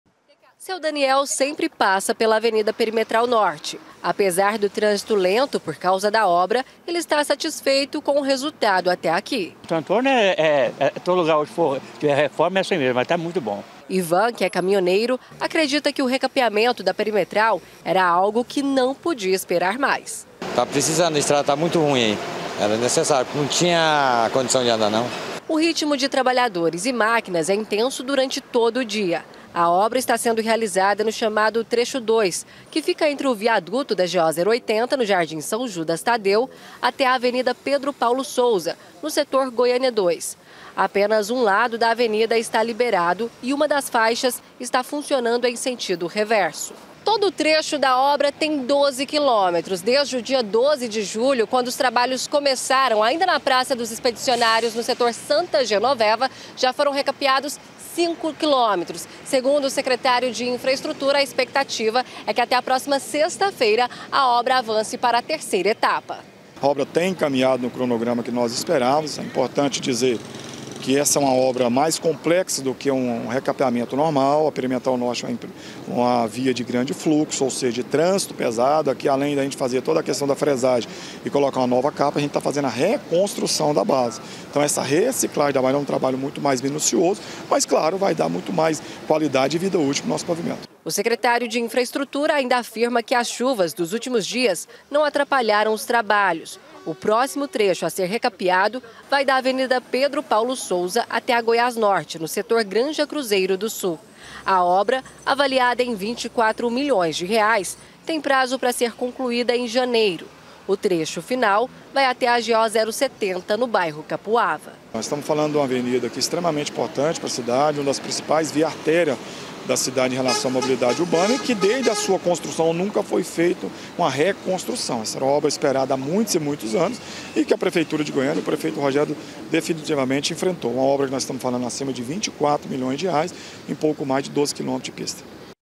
RITMO INTENSO: RECONSTRUÇÃO DA PISTA DA PERIMETRAL NORTE AVANÇA
A SEGUNDA ETAPA DO RECAPEAMENTO DA AVENIDA PERIMETRAL NORTE DEVE SER CONCLUÍDO AINDA ESSA SEMANA. A REDUÇÃO NO TRÂNSITO POR CAUSA DO FERIADO DE SETE DE SETEMBRO DEVE ACELERAR AINDA MAIS O RITMO DA OBRA. APESAR DOS TRANSTORNOS, MOTORISTAS APROVAM A REFORMA.